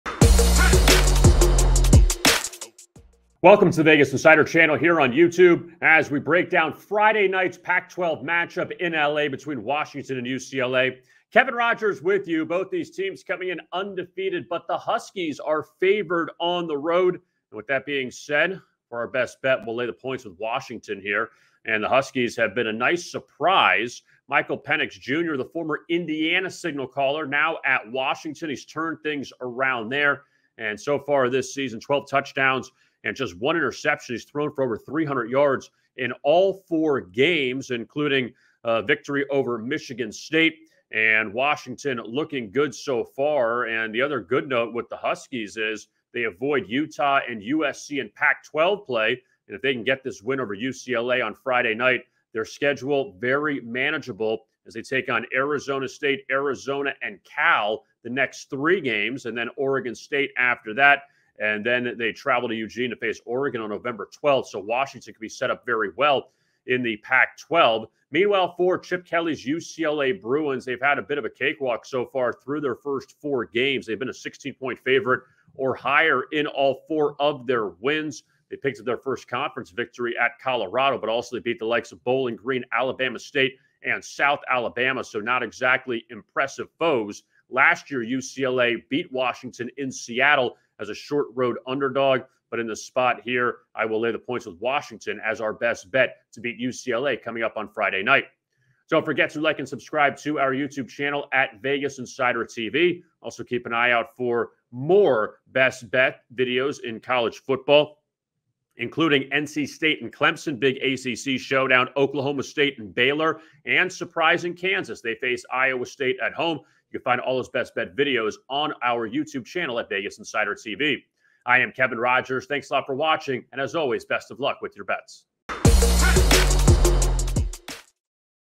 Washington vs. UCLA Predictions | College Football Week 5
Kevin Rogers breaks down the Pac-12 showdown on Friday September 30 in Los Angeles, California between the Washington Huskies and UCLA Bruins. Rogers hands out a side in this game as the Huskies are favored on the road against the Bruins.

TOPICS:
0:00 - Washington vs. UCLA
0:13 - Friday's Best Bet
0:25 - Washington's hot start
0:45 - UW's manageable schedule
1:09 - UCLA seeks 5-0 record
1:39 - Like and Subscribe